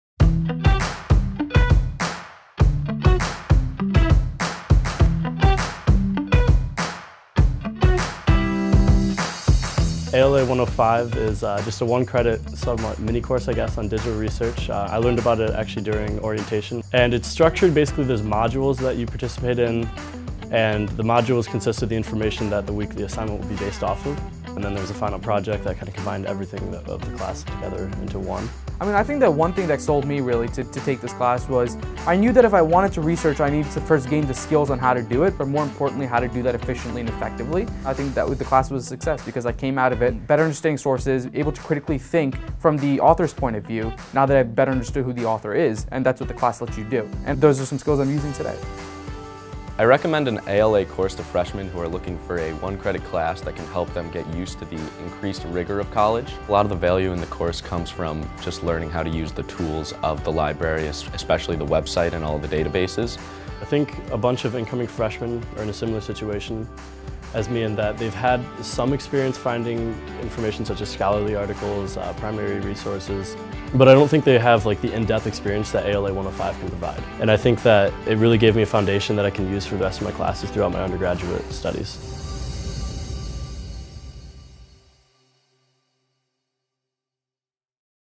ALA 105: Digital Research
University of Michigan students can sign up for ALA 105: Digital Research — a 7-week, 1-credit course that prepares them to succeed.